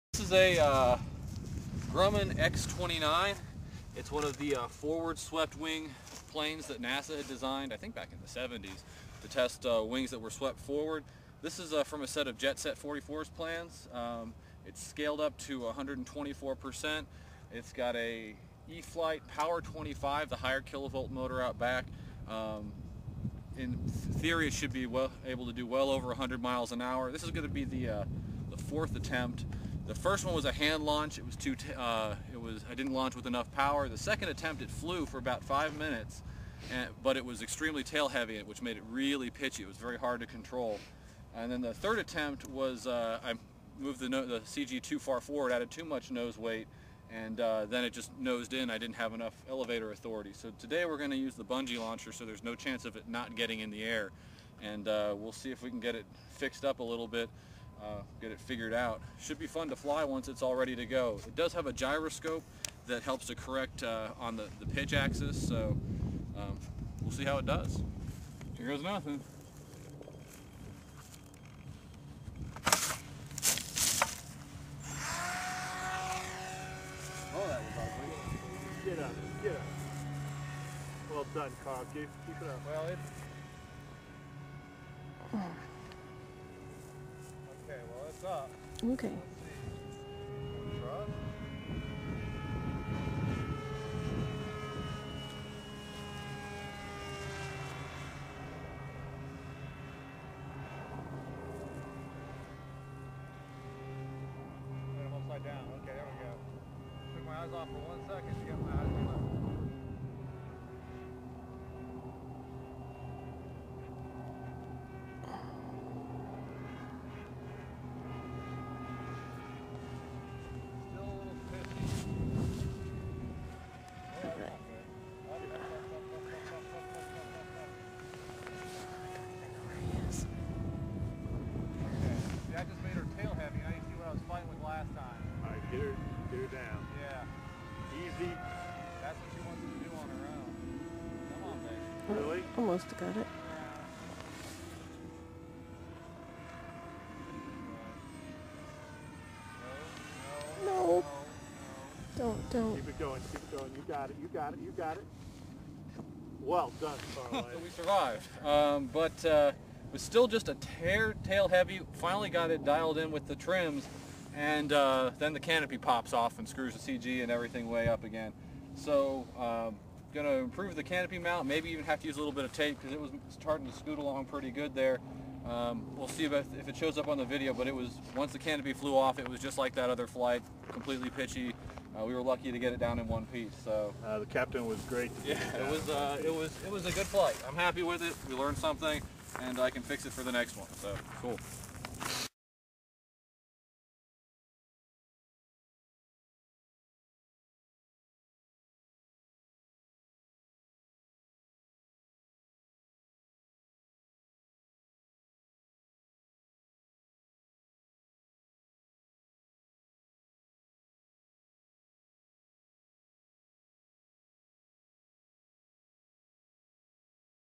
BIG RC Grumman X-29
This is the first video of my Grumman X-29 parkjet.

She was built from a set of Jetset44's plans, available on RC groups.  Power system is a e-flite power 25 with a 60amp esc and an APC 9x9e prop.  Battery is a 4cell 3000mAh.

Early tests show that she should fly well once I get the CG properly dialed in.  During this flight I lost the canopy, which induced a VERY tail heavy state and made her hard to control.  I'll get more video of this bird the next time we go out.